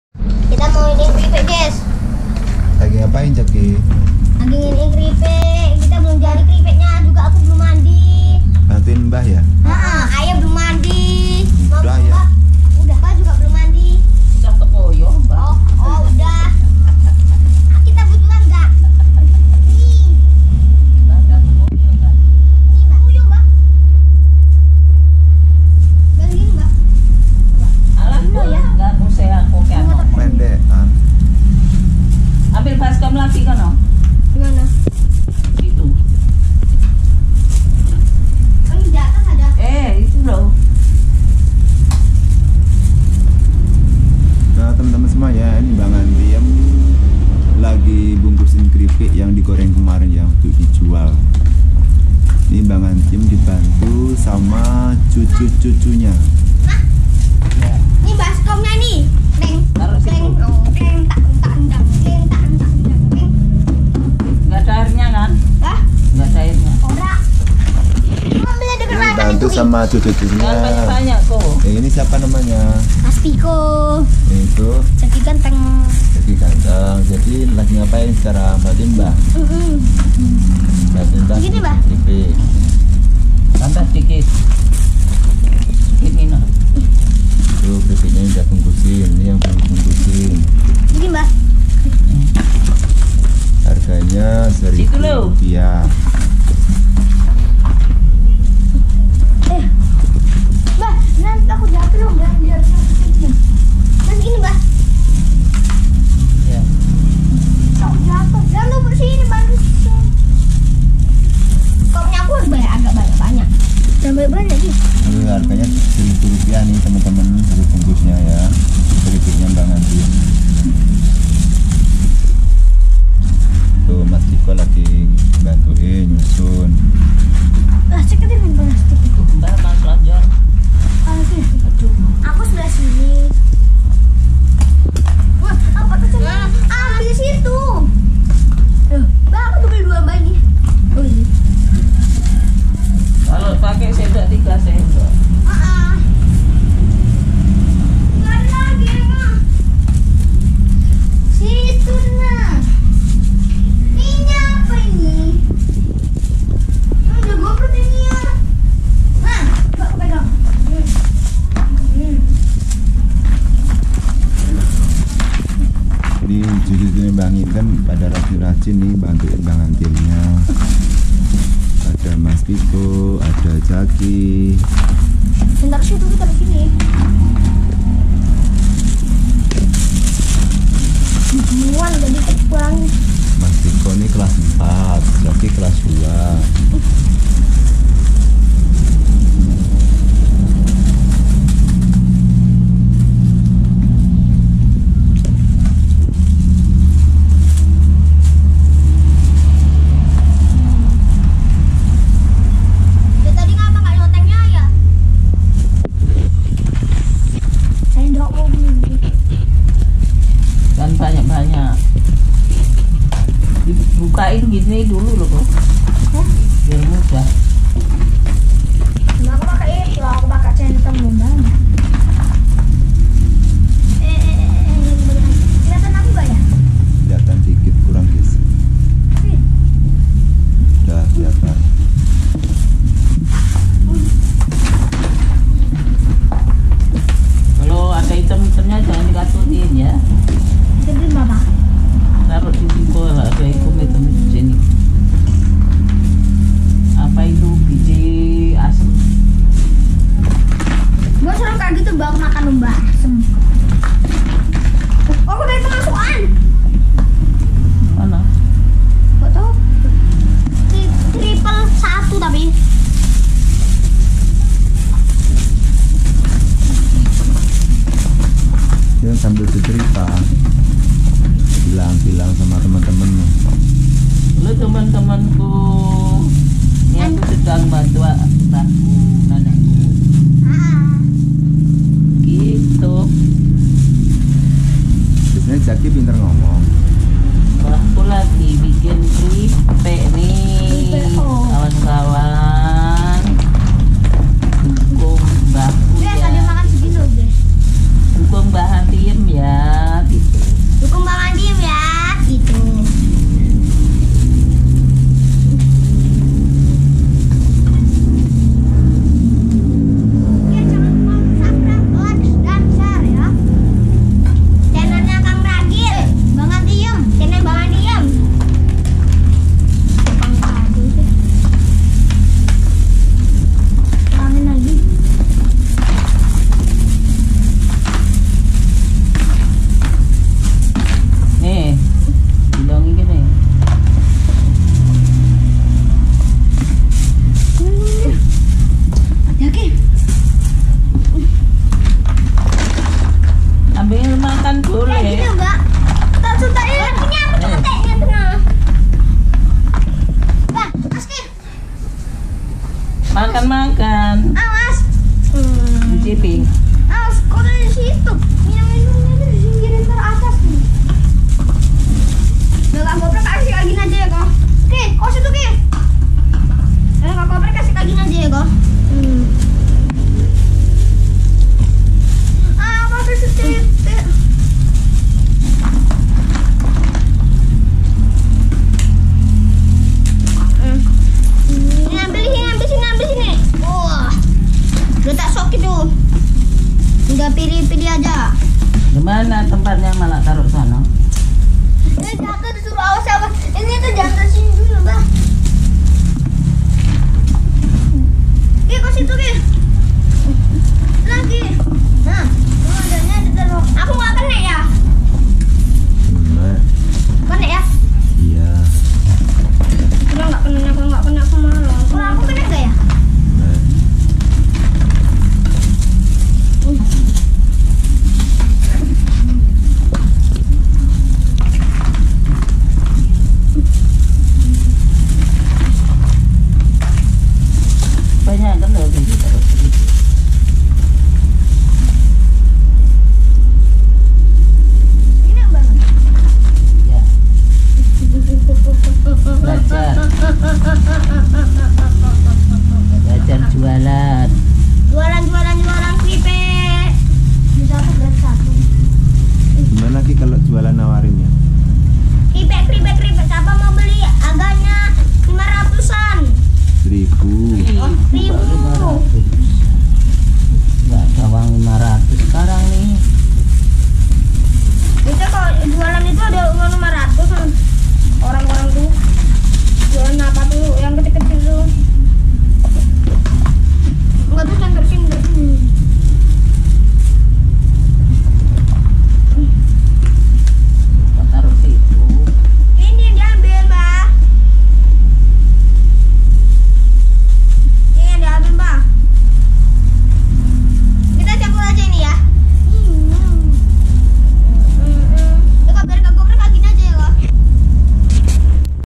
Cara Mbah Ngantiyem Packing Keripik Ubi Untuk Dijual
Pada video kali ini mbah ngantiyem sedang packing keripik ubi untuk dijual. Mbah ngantiyem packing keripik ubi dibantu kedua cucunya. Bagaimana cara mbah ngantiyem packing keripik ubi, yuk simak videonya.